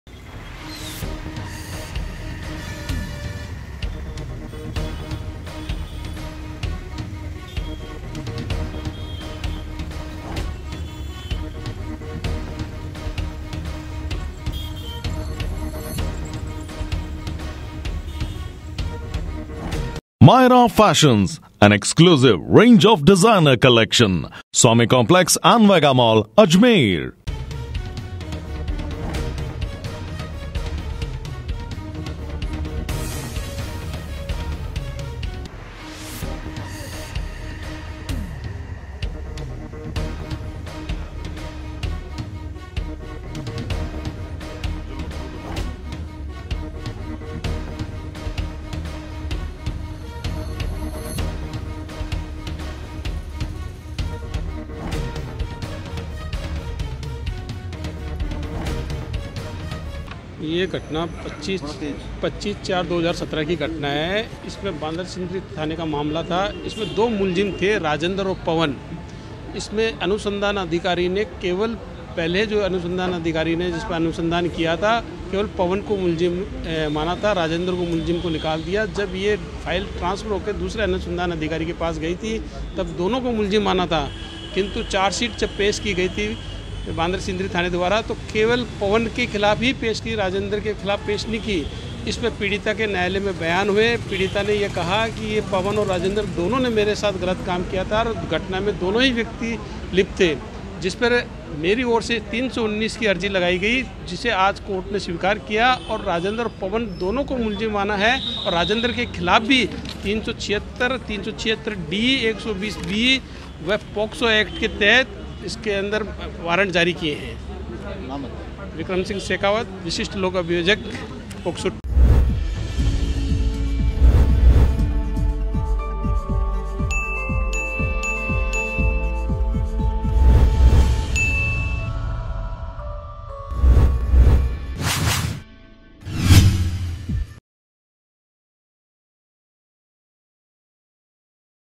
AJMER NEWS | गुरुवार को पोक्सो एक्ट न्यायालय ने पीड़िता की शिकायत पर | MTTV INDIA
गुरुवार को पोक्सो एक्ट न्यायालय ने पीड़िता की शिकायत पर
दूसरे आरोपी को मुल्जिम बनाने के दिए निर्देश
बांदर सिंदरी थाना पुलिस को दिए आरोपी राजेन्द्र को  मुलिजम बनाने के निर्देश
25 अप्रैल 2017 को पीड़िता ने अश्लील वीडियो बनाकर दुराचार का मुकदमा कराया था दर्ज